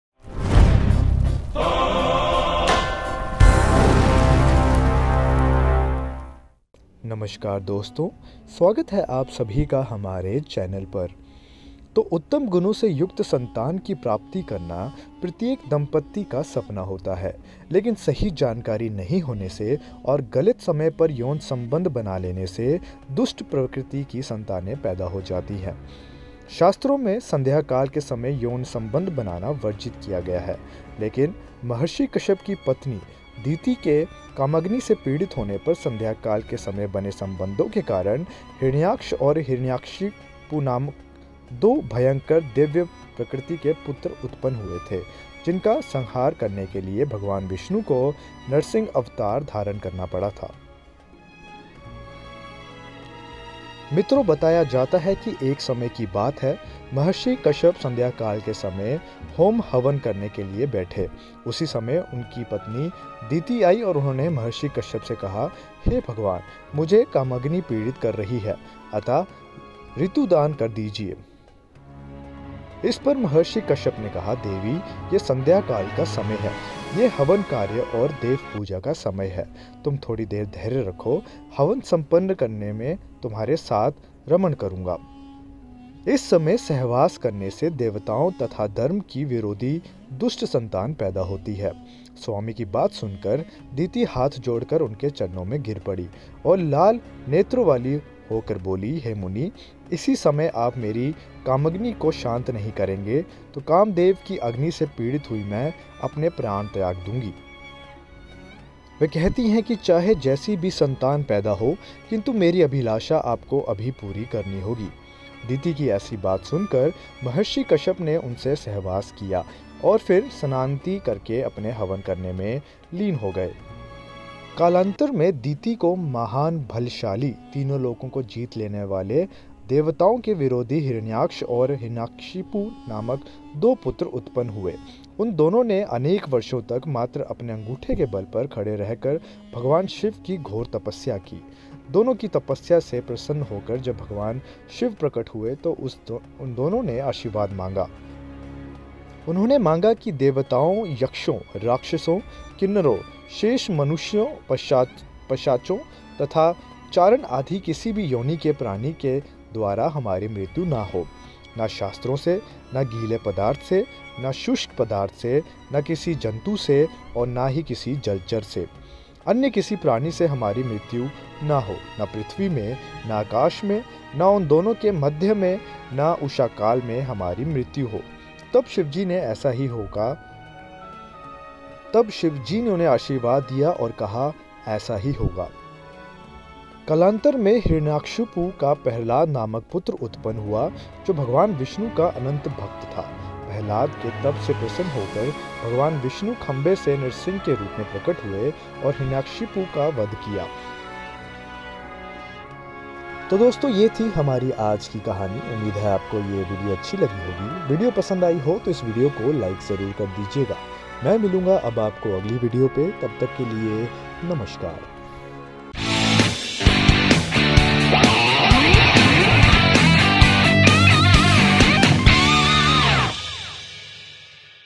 संध्या काल में यौन संबंध बनाने से पैदा होती है दुराचारी संतानें | इससे जुड़ी है एक पौराणिक कथा
Watch the video to know about what should be done for a Cultured child.